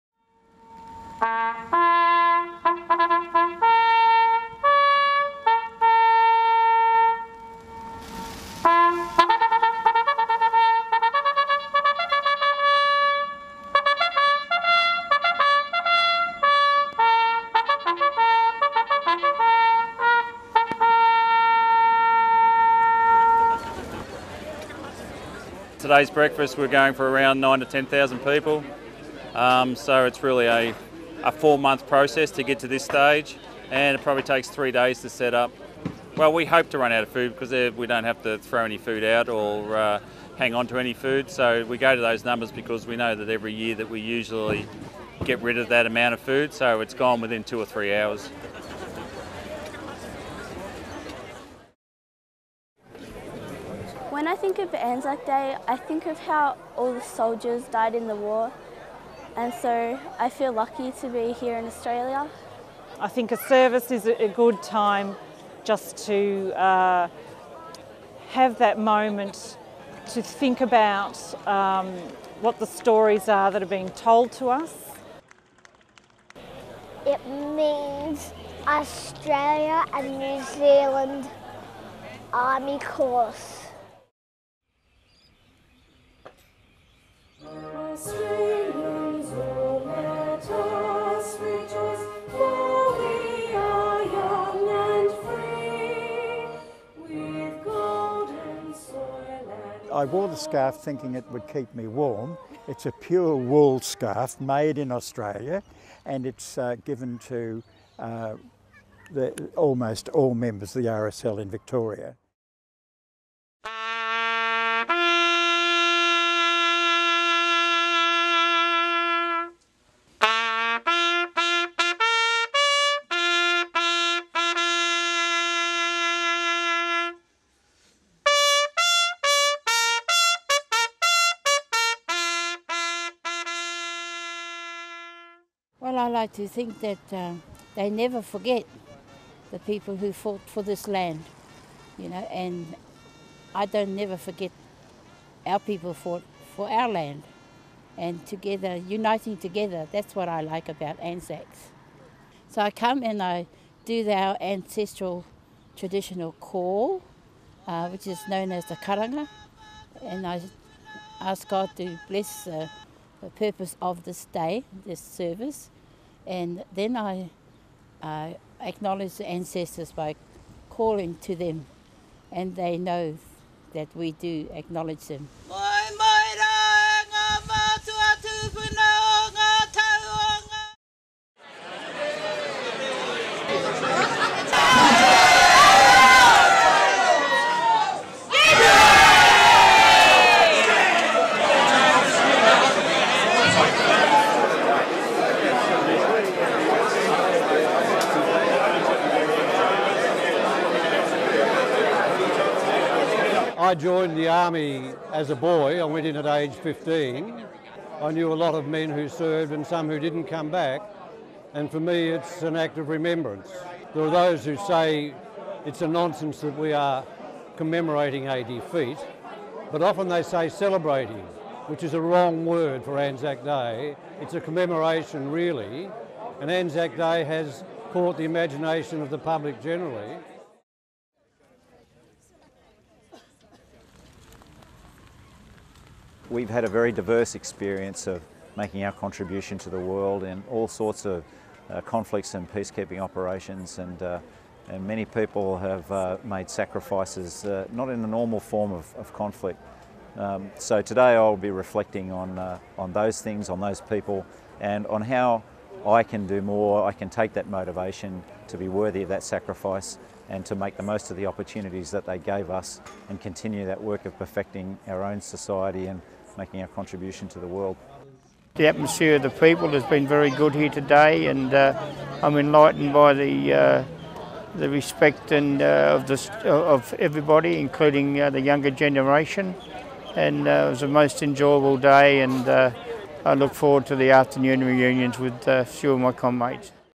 Anzac Day around Melbourne || PSC x The Age
On Anzac Day, PSC PJ students covered commemorative events at various locations throughout Melbourne. The resulting multimedia video, edited by Fairfax Senior Producer Tom McKendrick, was featured on the home page of The Age website.

Want to work on projects like this?